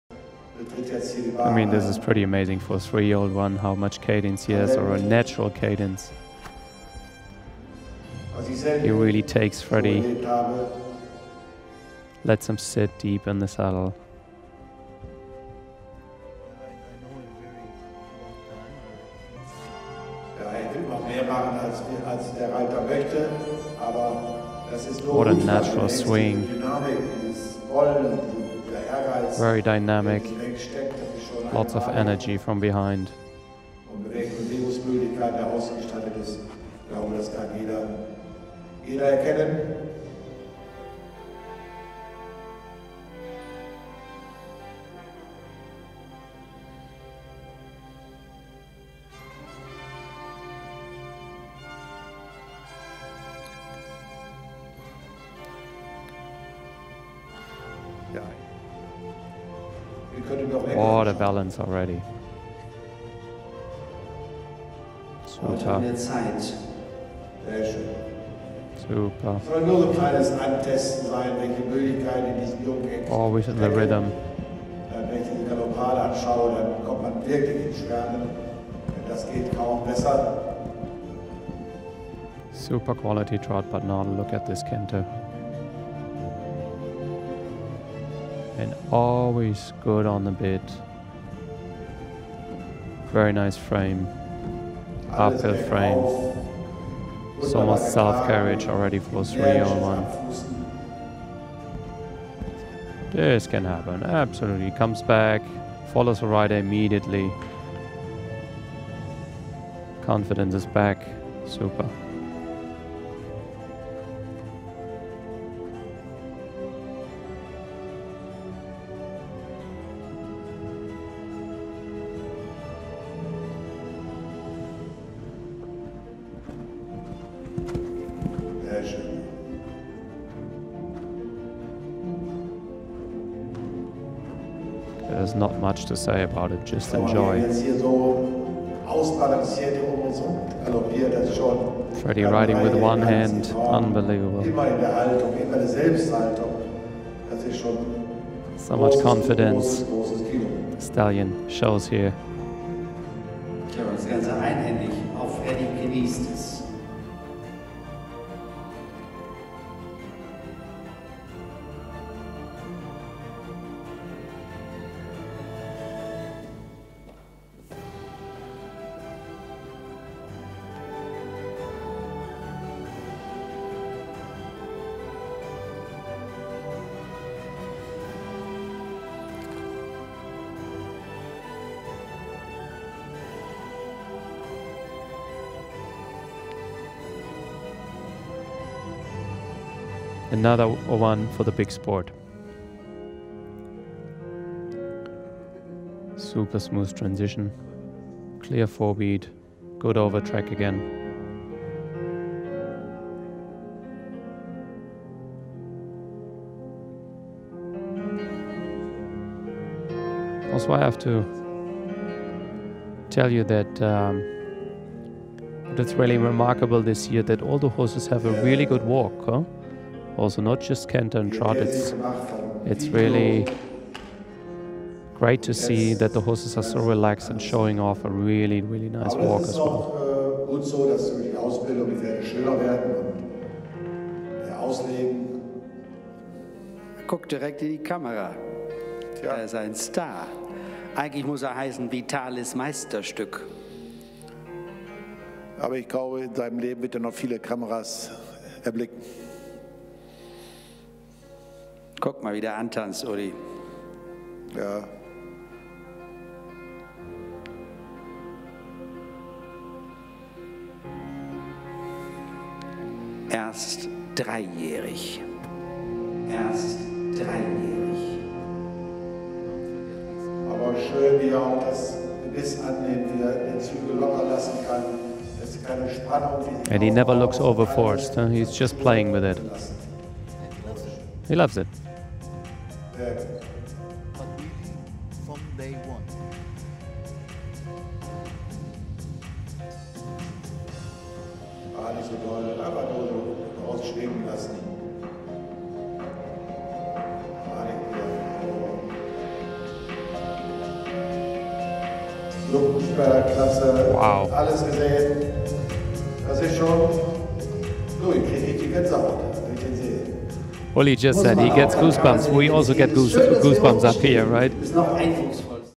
Virtual Story Live Presentation ENG
P.S.I. Dressage Collection 2020
Catalog No. 20 Virtual Story (Vitalis - Don Primero)